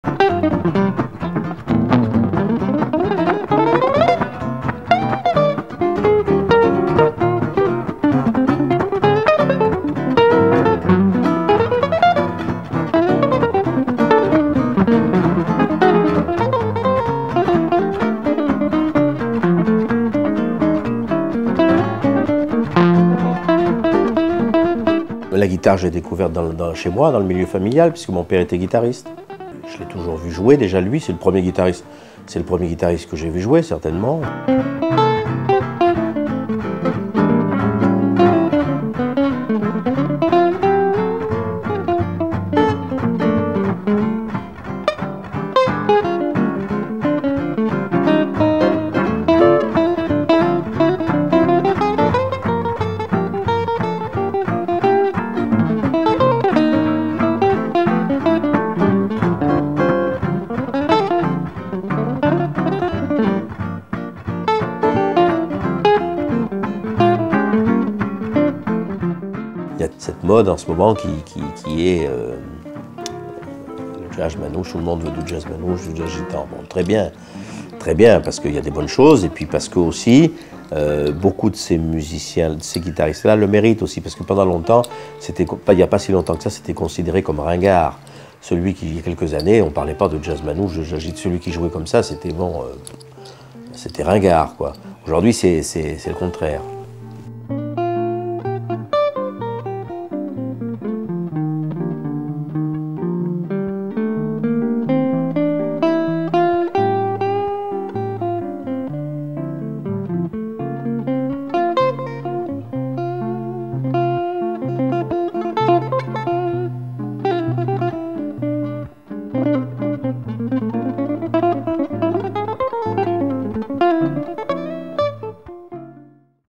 Mondomix présente : Christian Escoudé
Lorsque l'on parle références avec un guitariste de jazz manouche, on retombe invariablement sur Django Reinhardt, Django Reinhardt et certaines fois aussi un peu Django Reinhardt. Avec Christian Escoudé, on retrouve bien sûr ce lien de parenté légitimement revendiqué, mais pas seulement.

Ce guitariste né en 1947 à Angoulême, d'un père tzigane et dune mère charentaise, a commencé son apprentissage en copiant son géniteur et son oncle. A 15 ans, il devient professionnel en jouant dans les bals de sa région. Pendant cette période, il est influencé par Charlie Parker, Coleman Hawkins et forge son jeu dans l'orchestre de la base militaire américaine d'Angoulême.

En 1976, Christian Escoudé remporte le prix Django Reinhardt. Devenu un musicien de jazz reconnu, il enchaîne les collaborations. Il part sur les routes en 1980 avec John McLaughlin, entre dans l'orchestre de Martial Solal en 81 et crée en 85 le Trio Gitan, avec Boulou Ferré et Babik Reinhardt. Il réorchestre les compositions de Django pour un big band et lance en 2004, le Nouveau Trio Gitan avec Martin Taylor et David Reinhardt.

Filmé à La Cité de la Musique de Paris.